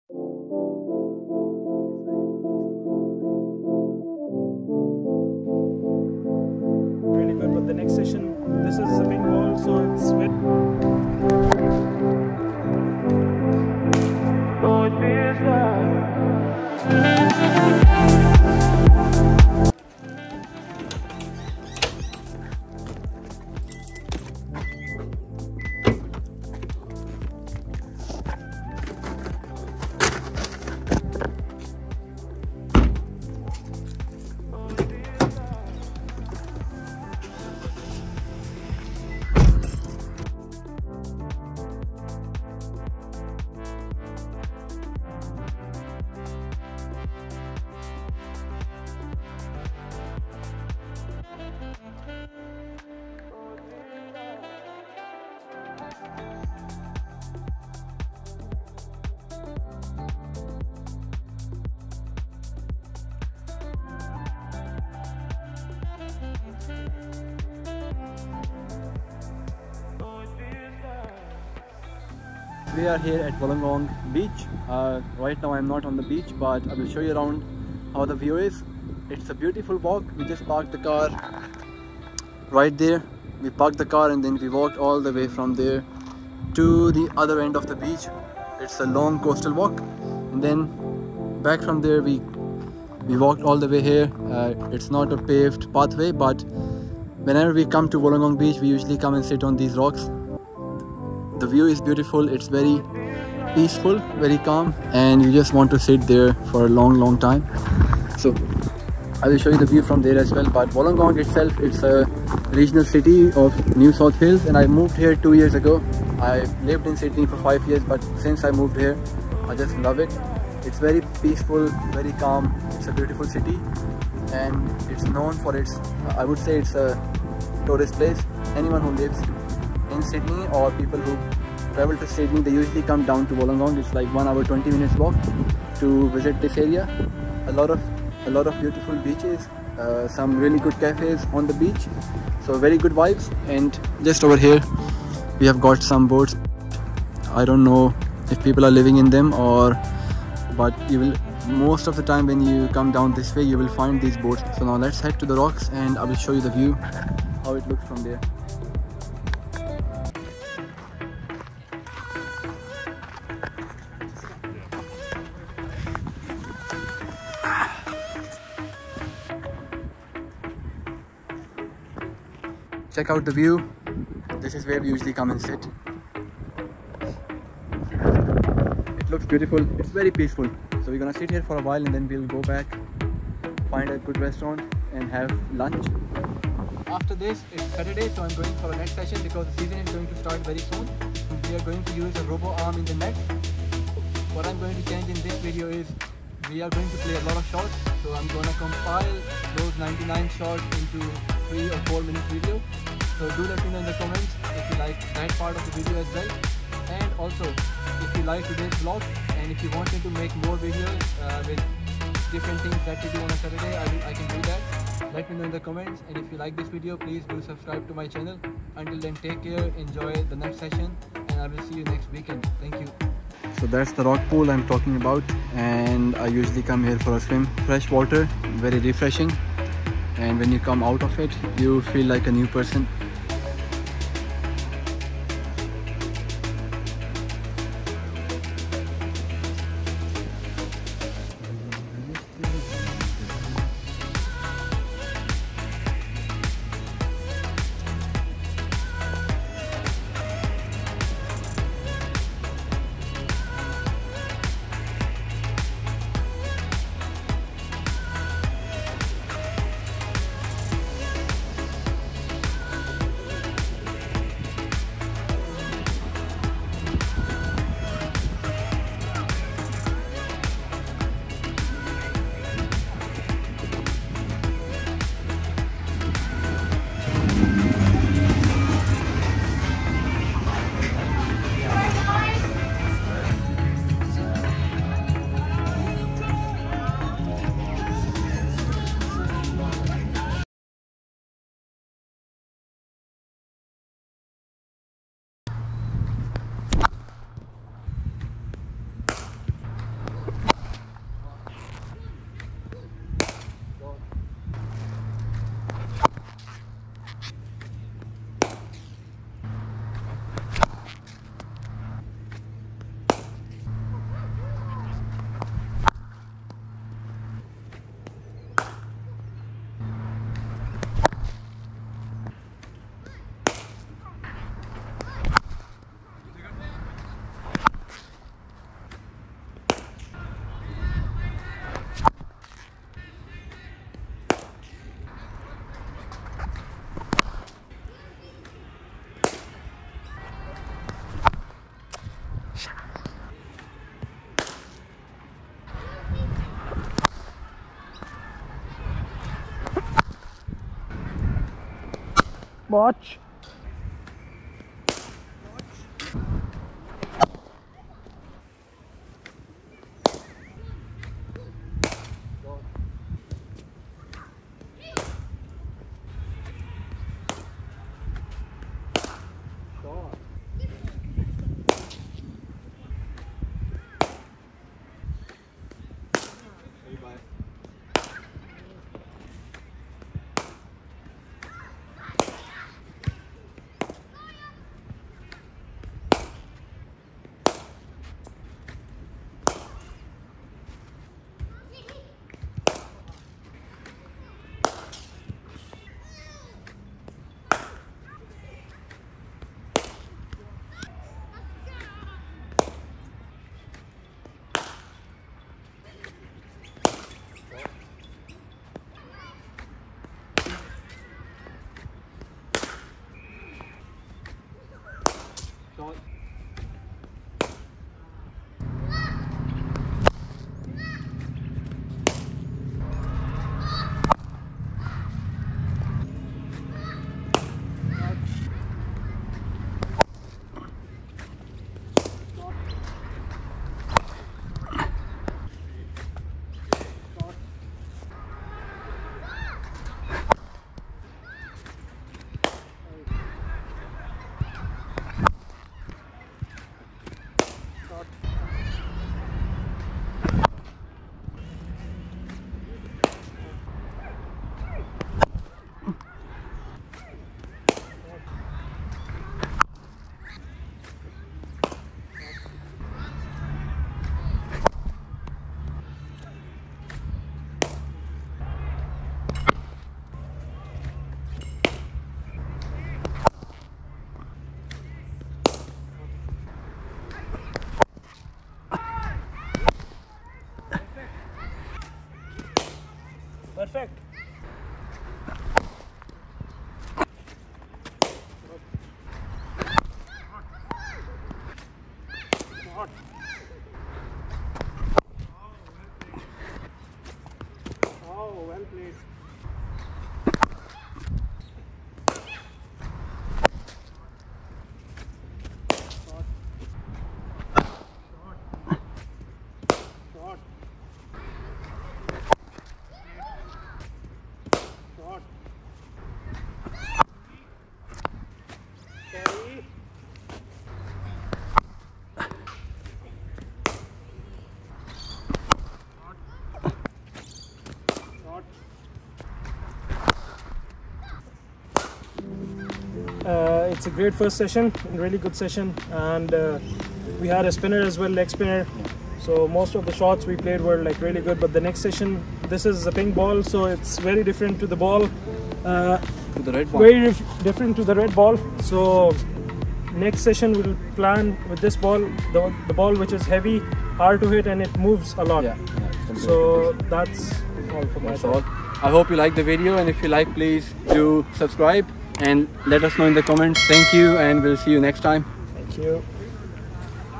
WOLLONGONG BEACH TOUR & 99 SHOTS in CRICKET NET SESSION - Gopro Cricket Helmet Cam
WOLLONGONG BEACH TOUR AND CRICKET NET SESSION ON a weekend off when we don't have a cricket match.